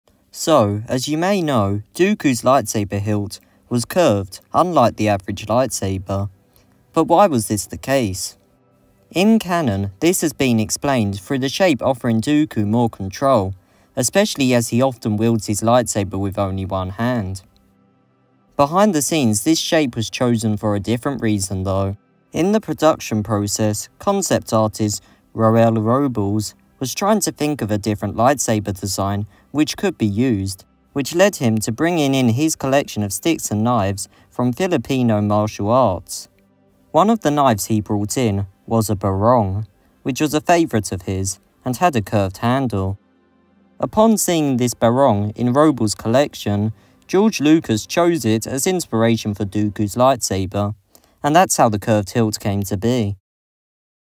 Why was Dooku’s lightsaber curved? (Canon)
In attack of the clones we were introduced to count dooku who unlike those before him had a unique curved lightsaber hilt but why was this the case. In this video I’m covering both the canon and real world reason for this shape. I hope you enjoy